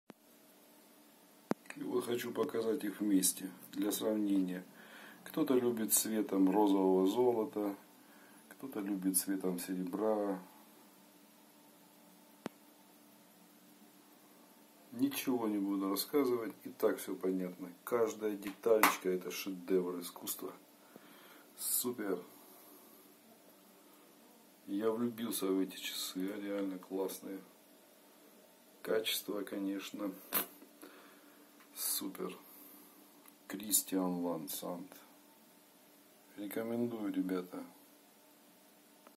💥В наличии.💥 Мужские часы хронограф бренда Christian Van Sant. 2 цвета.
💥В наличии.💥 Мужские часы хронограф всеми любимого и уважаемого американского бренда Christian Van Sant.
Предлагаем вам 2 цвета:
Синий с розовым золотом и
Черный с полированным серебром
⌚️Великолепная модель 45 миллиметрового диаметра в стиле бренда Инвикта только меньшего размера. Инвикта такие модели делает диаметром оверсайз - очень большими. Теперь, для любителей спорт-стиля, появилась возможность приобрести супернавороченную модель среднего размера в исполнении культового бренда Christian Van Sant.
🏆Часики сделаны с тончайшей точностью в каждой мелкой детальке. Любовь к этой модели приходит от первого взгляда на картинку.
Сочетание цветов  притягивает взгляды.
👍🏊Всё в этих часах надёжно: минеральное стекло, японский кварцевый механизм с хронографом, сталь с технологичной защитой PVD, водозащита 100м, крепкий и эластичный силиконовый ремешок, документы и годовая гарантия.
💰Стоимость на официальном сайте Christian Van Sant 895 $. В магазинах города таких нет.
👉Успей купить.
🚙 Доставка по миру через СДЭК
Рекомендую 👇
👍 --- @massaj_jzt - массаж лечебный